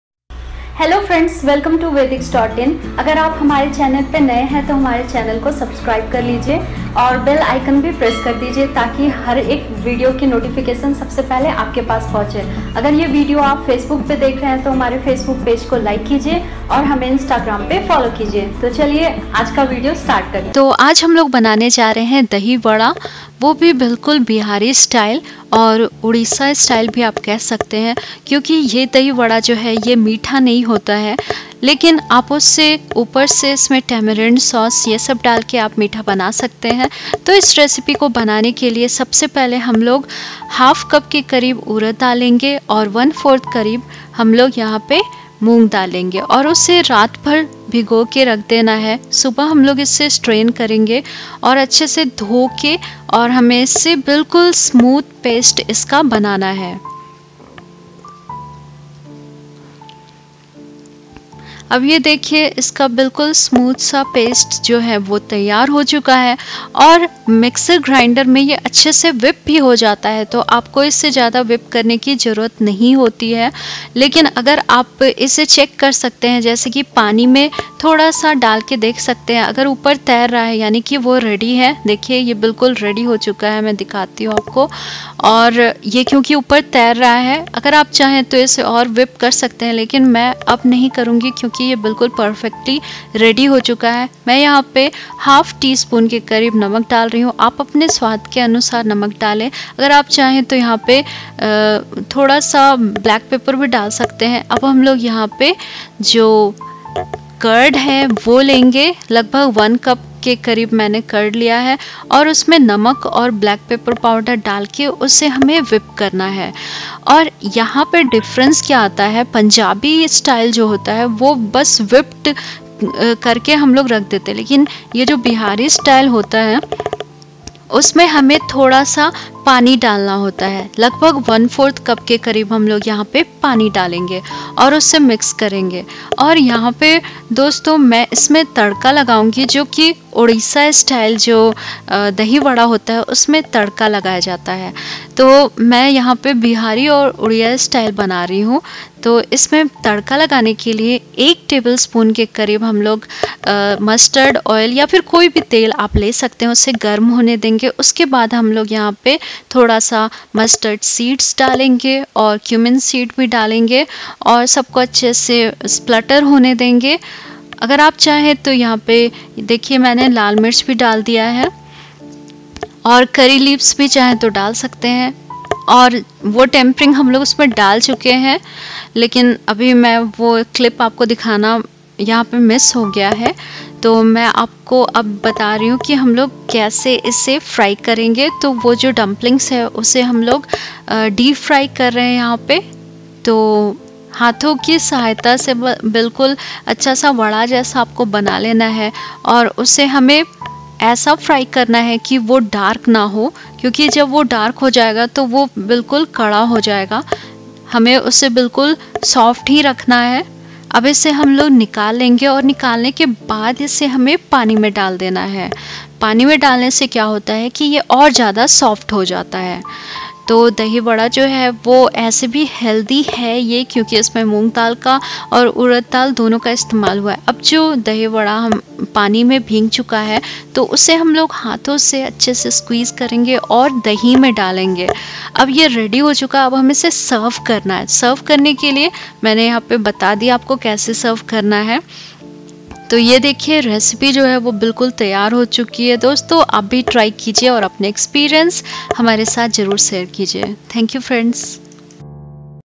Snacks, starter | Dahi Vada For Dahi Vada Aloo Dum | Dahi vada Recipe | Dahi vada Odisha style
Ingredients
½ Cup White lentils
¼ Cup Mung lentils
½ tsp salt
¼ tsp Black pepper poweder
¼ tsp Roasted cumin powder
1 tbsp Tamarind chutney
¼ tsp Kashmiri red chilli powder
1 tbsp Chopped coriander leaves
1 tbsp Mustard seeds
1 Cup Curd
¼ Cup Water
1 tsp Mustard seeds
½ tsp Cumin seeds
1 tsp Curry leaves
1 Piece Dried red chilli